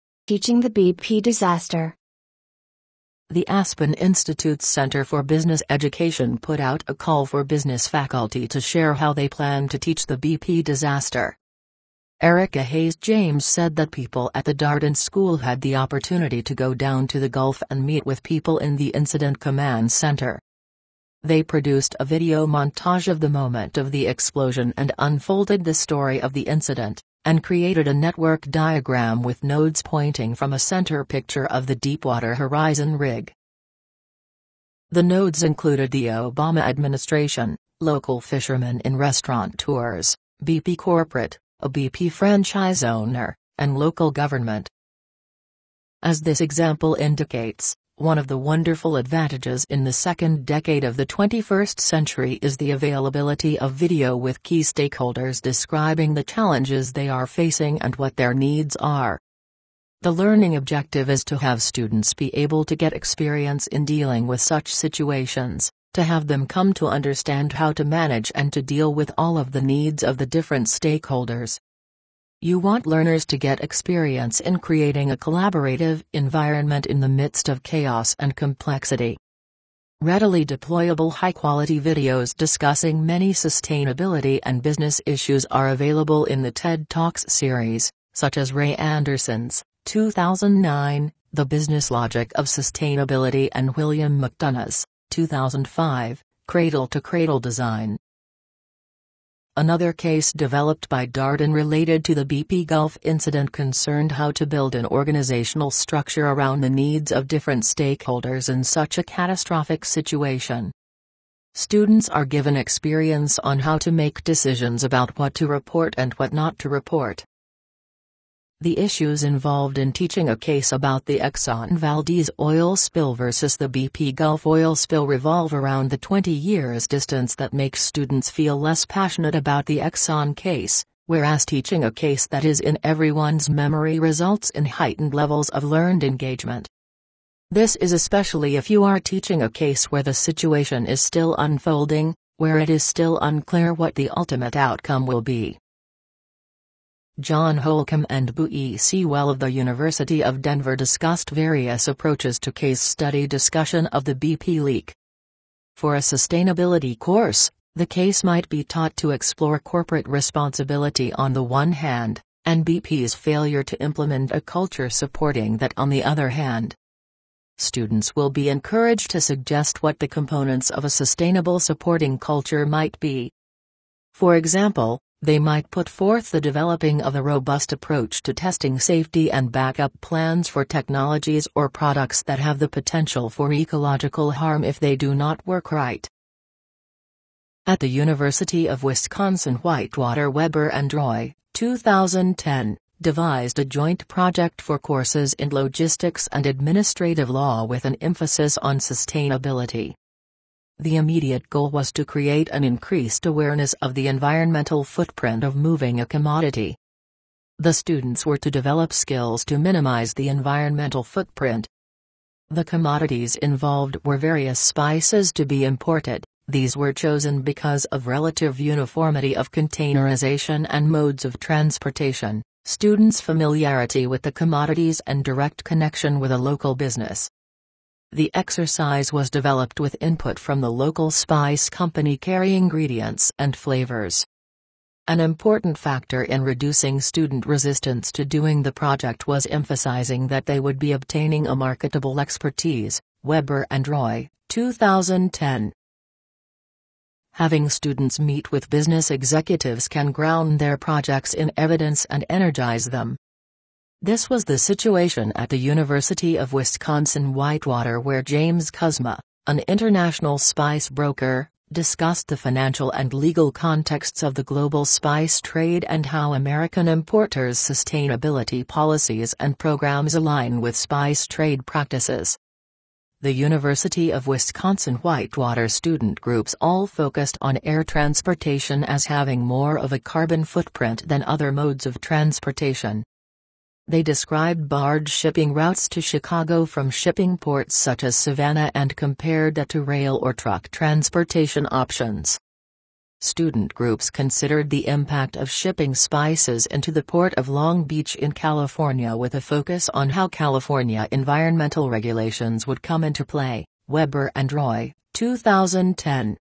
Teaching the BP Disaster
Erika Hayes James said that people at the Darden School had the opportunity to go down to the Gulf and meet with people in the incident command center. They produced a video montage of the moment of the explosion and unfolded the story of the incident, and created a network diagram with nodes pointing from a center picture of the Deepwater Horizon Rig. ...